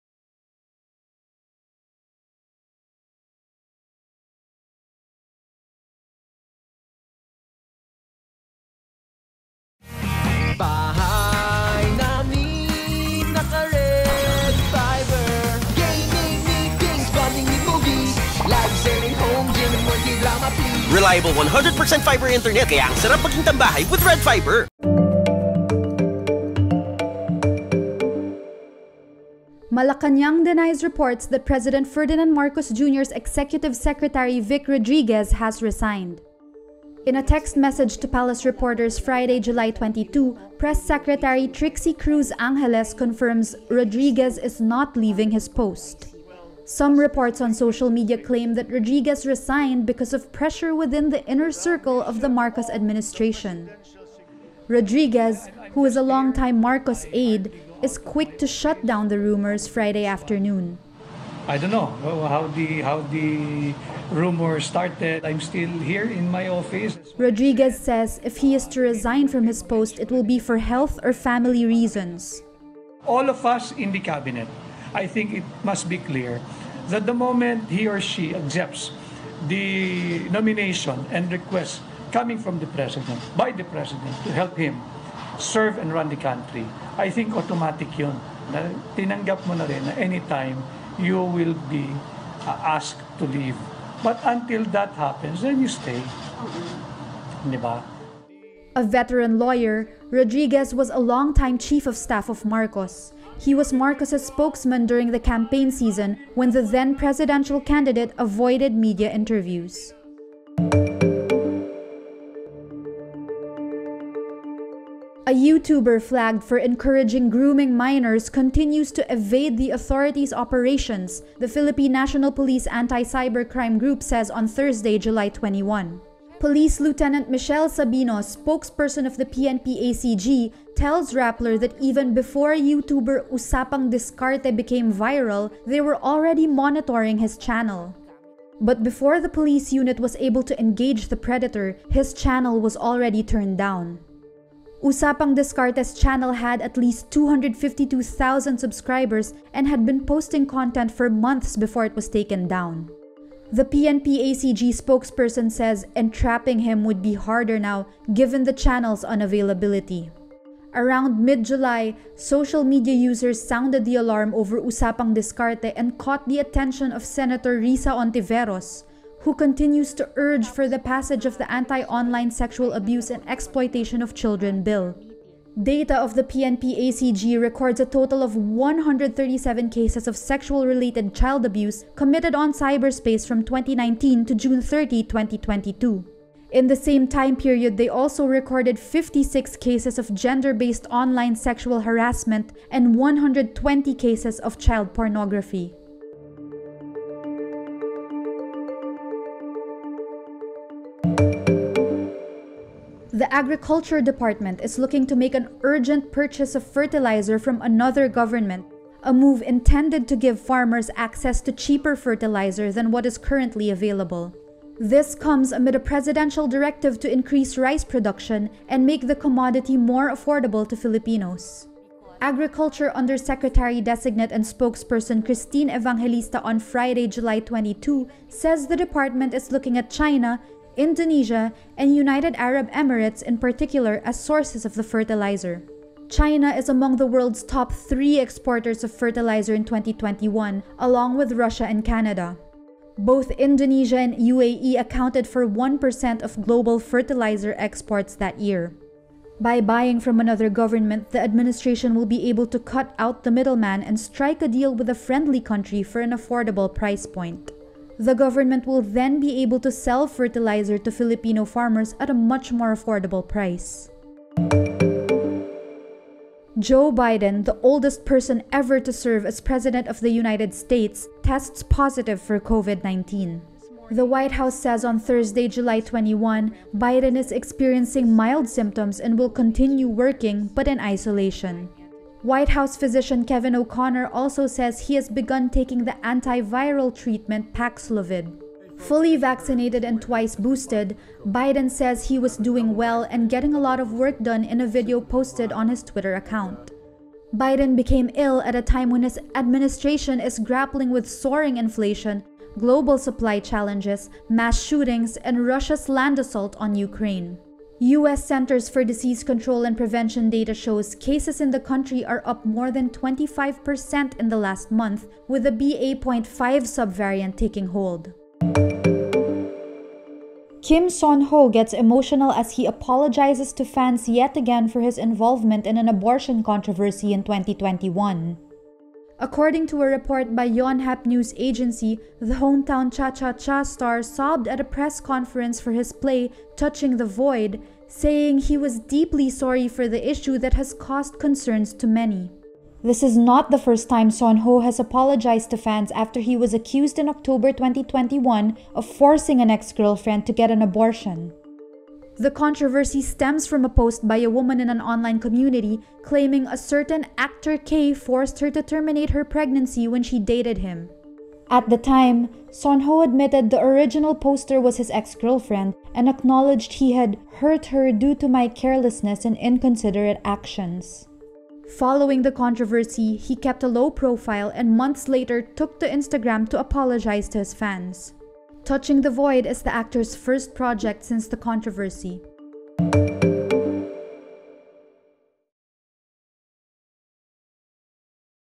Vic Rodriguez denies resignation rumors | Evening wRap
-‘I am still here’: Executive Secretary Vic Rodriguez dismisses resignation rumors
-YouTuber encouraging child sex abuse continues to evade authorities – PNP
-Philippines studying government-to-government fertilizer deal with China, Indonesia, UAE
-Biden tests positive for COVID-19, has mild symptoms, working in isolation
-Kim Seon-ho gets emotional as he apologizes anew for abortion controversy